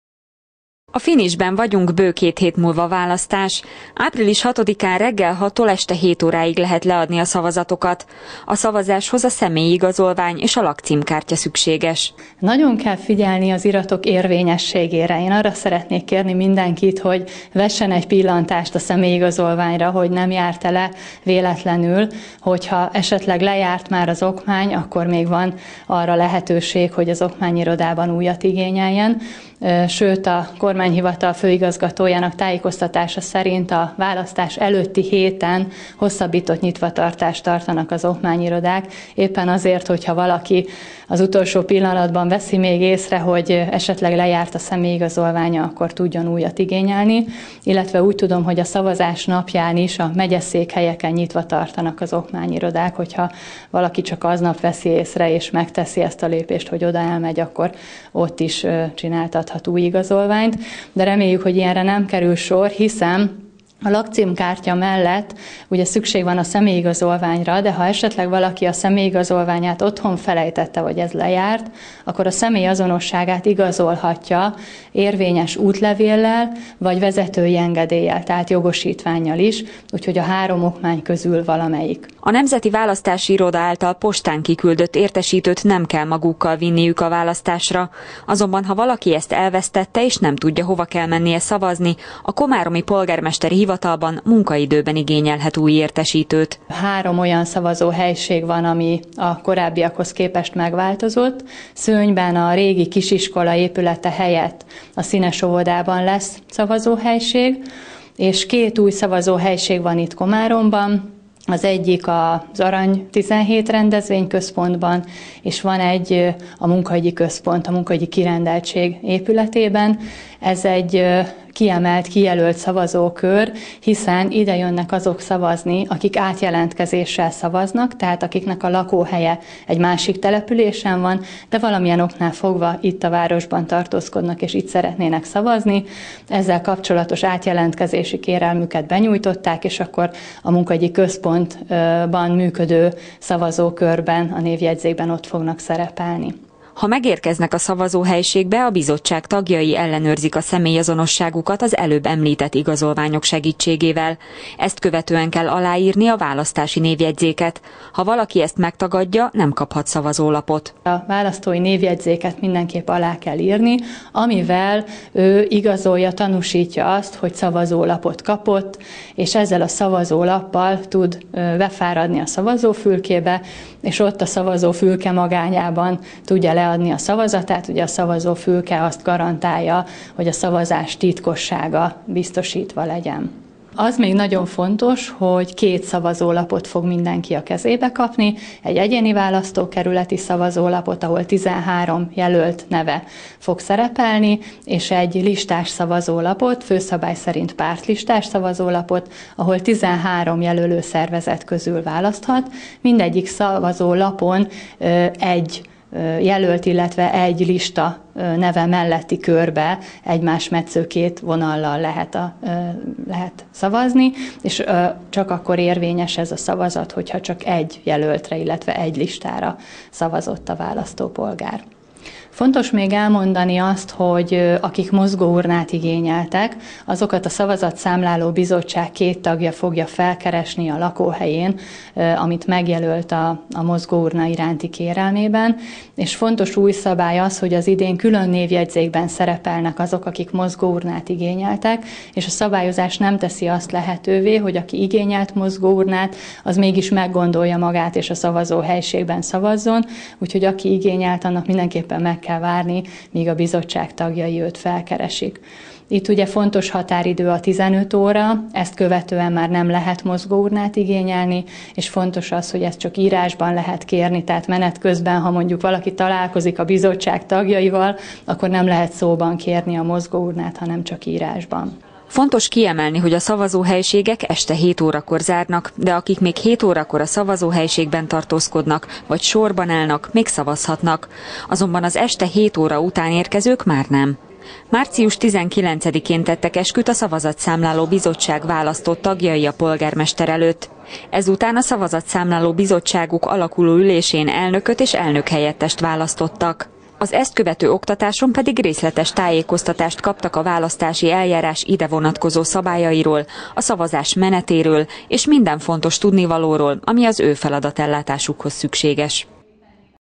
Választási információk
Milyen dokumentumok szükségesek a voksolásnál, hogyan zajlik a szavazás menete - többek közt erről is tájékoztat dr. Ströcker Renáta jegyző.